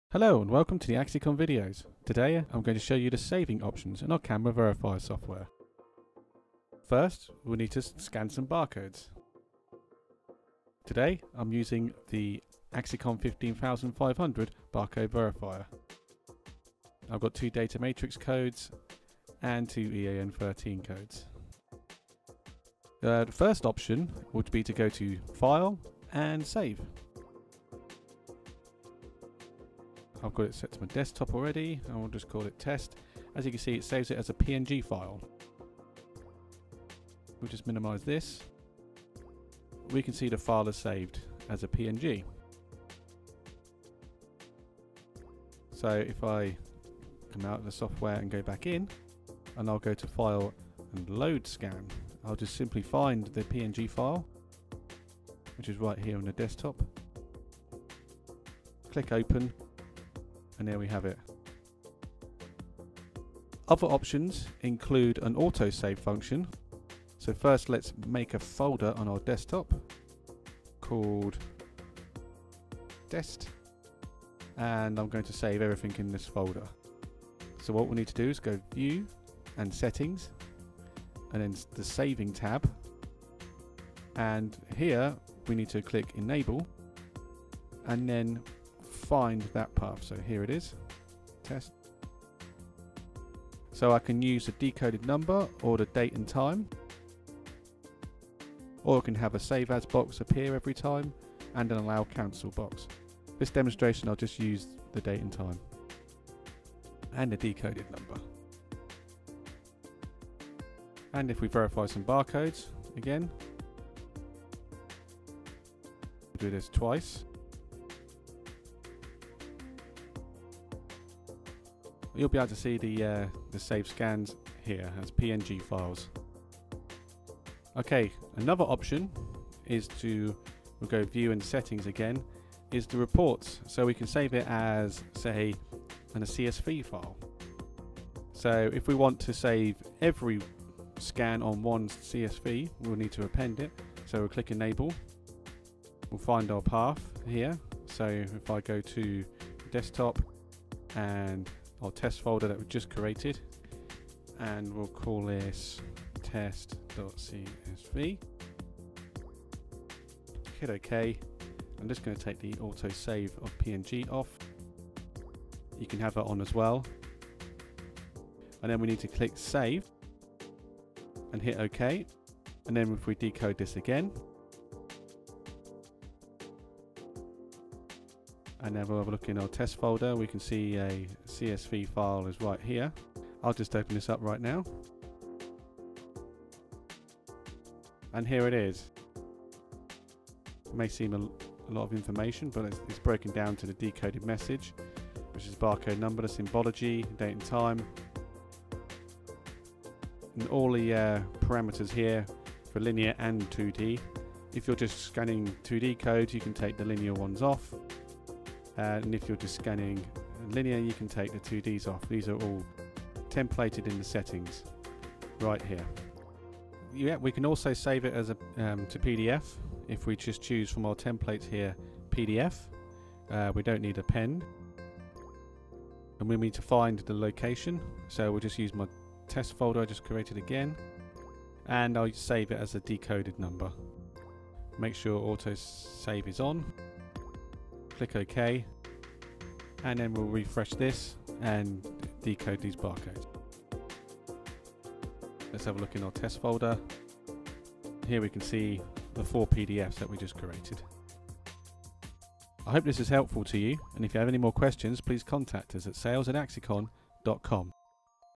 15500 barcode verifier saving options
15000 series barcode verifier software. Saving options including:

Manual saving
Auto saving
Report saving